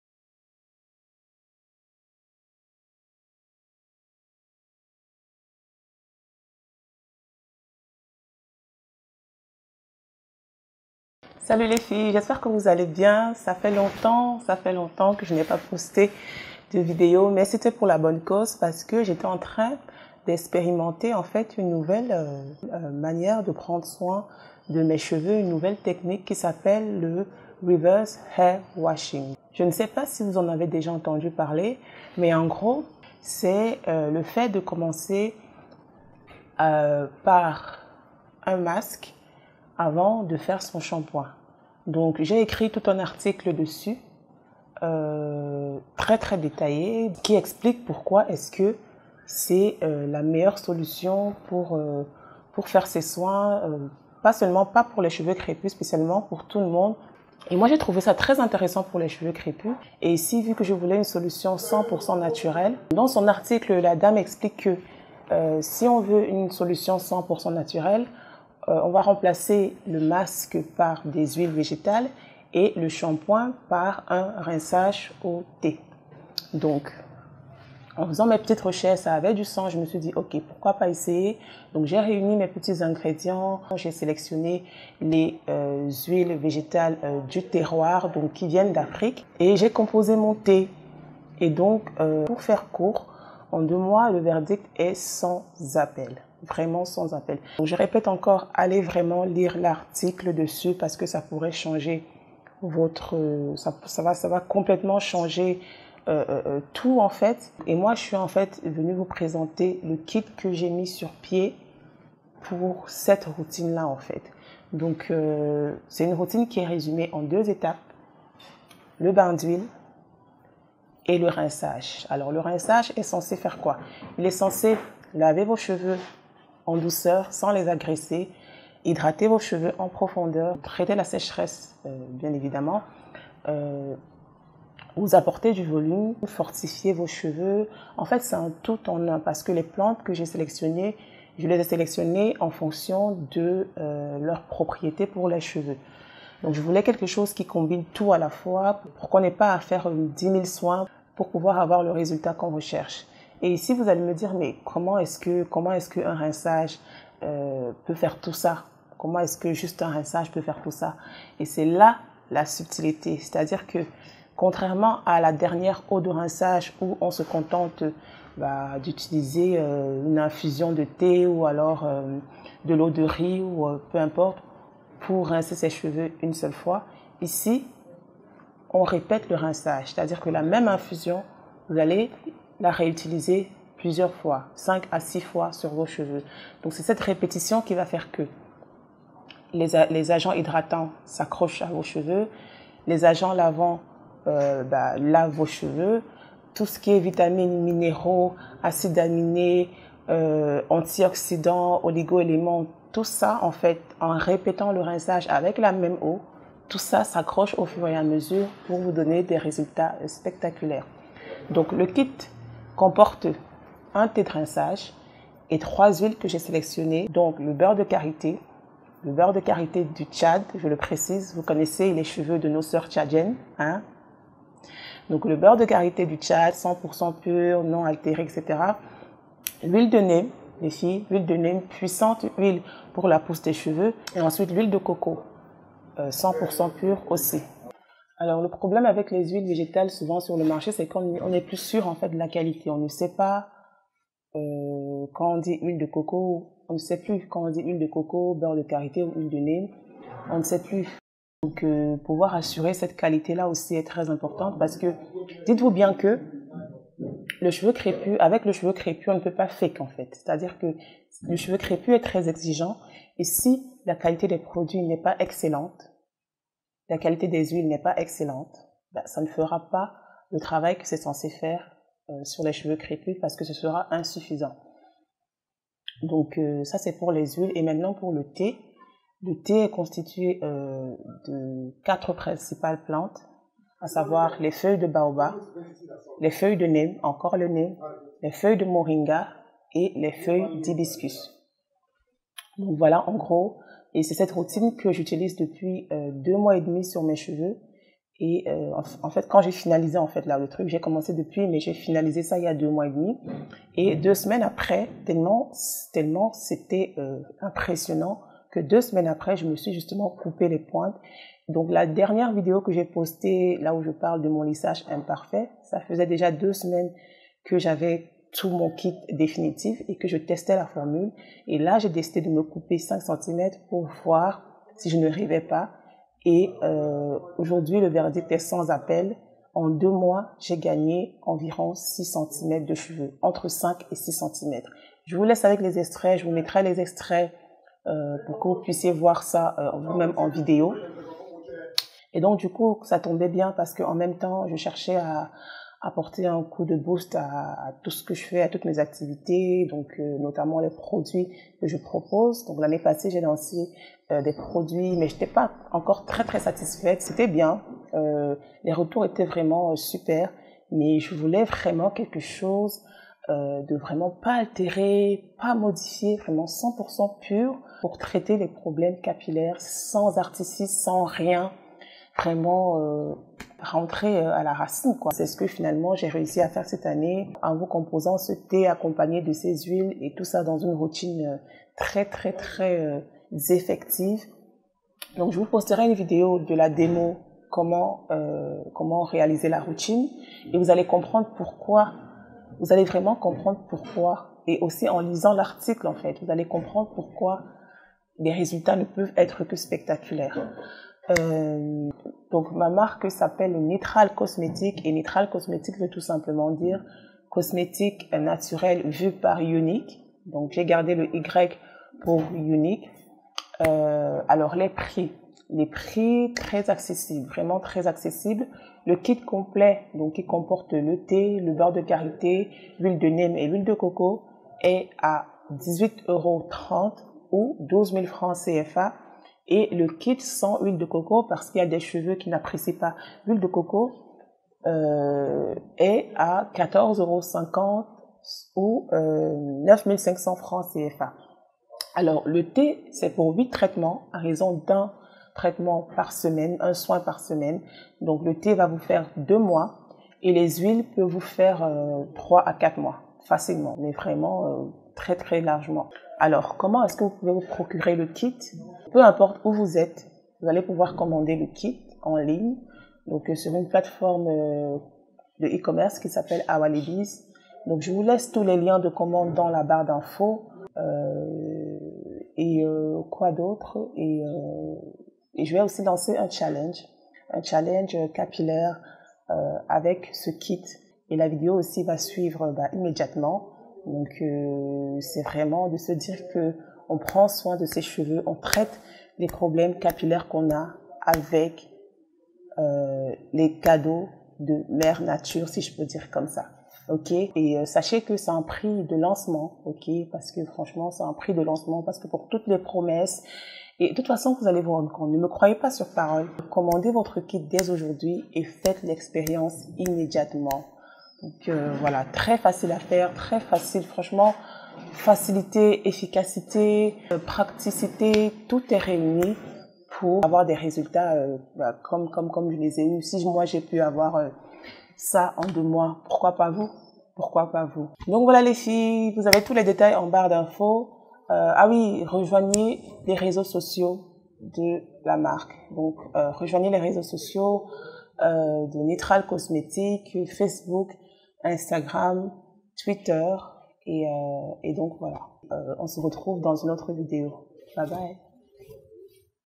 Comment J'ai Gagné 6 cm en 2 mois Grâce au "Reverse Hair Washing"
Téléchargez votre guide capillaire gratuit
Commandez directement vos produits
Visitez la boutique Facebook
Rejoignez Nytral Cosmetics sur les